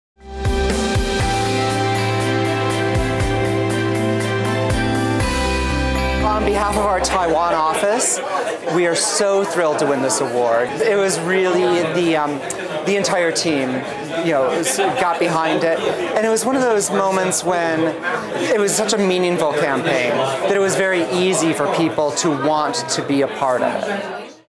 The 2017 Gold Standard Award for NGO Engagement: Golin Taiwan
The 2017 Gold Standard Award for NGO Engagement was won by Golin Taiwan for its work raising awareness of HIV testing and prevention.